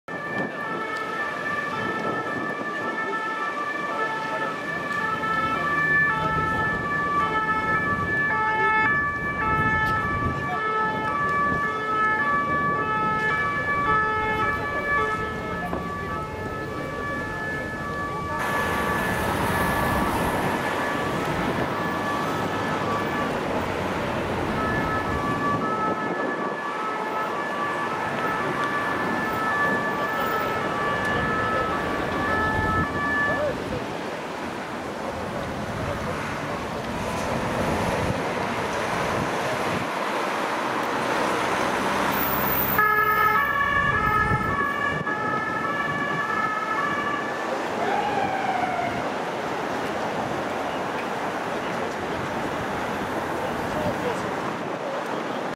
Convoy of the Ambassador U S in Paris
Convoi de  Madame l'ambassadrice des États-Unis en France et à Monaco   , lors de la fête nationale  le 14 Juillet 2019